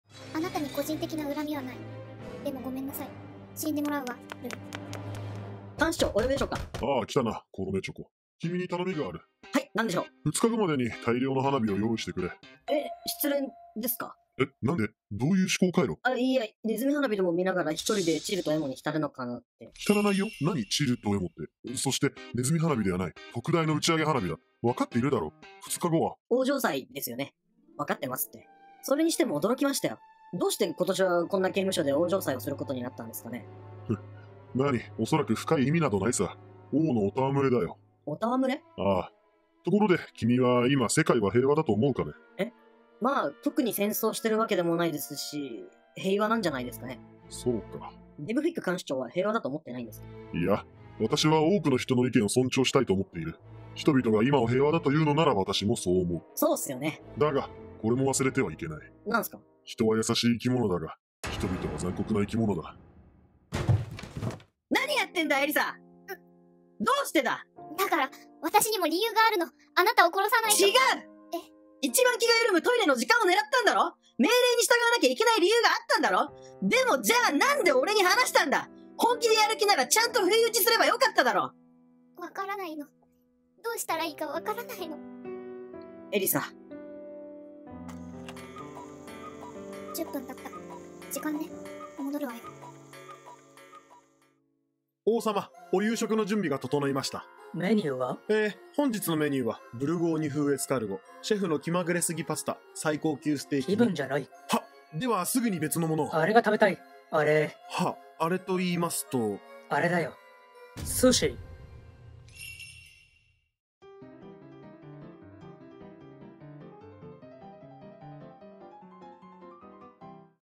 【アニメ】懲役200年の囚人/第4話
Twitterにちょこちょこ
設定資料的なものも
公開していこうと思っているので

さて、ここからが大変だぞ、ルイ達。

登場キャラクター
・ルイ 19歳
・エリサ 22歳
・デブフィック看守長 55歳
・コロネチョコ 25歳
どうも、ピュティフィです。
ヘンテコな日常を描いてます。